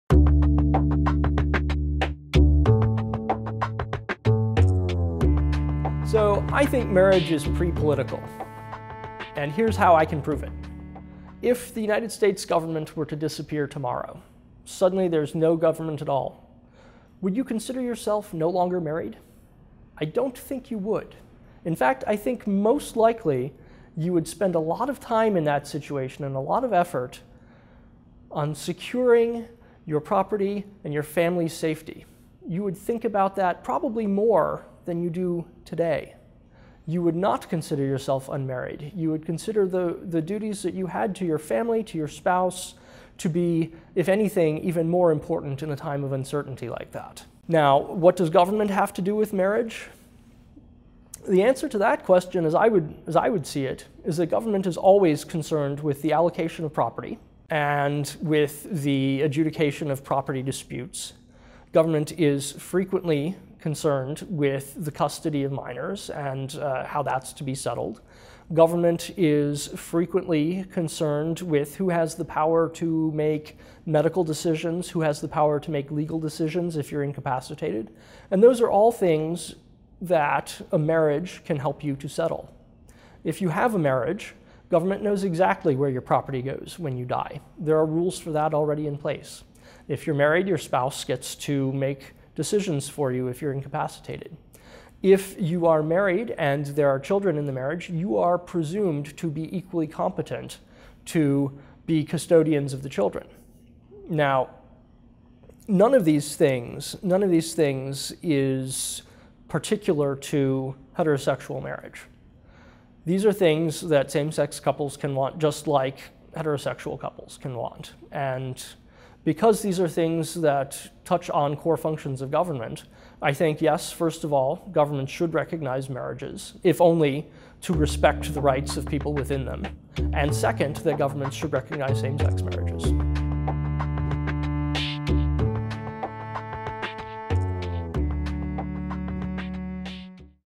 Should governments regulate marriage? (with Jason Kuznicki) - Libertarianism.org
Jason Kuznicki discusses the pre-political nature of marriage and  government’s role in the institution.

Produced by Mark McDaniel.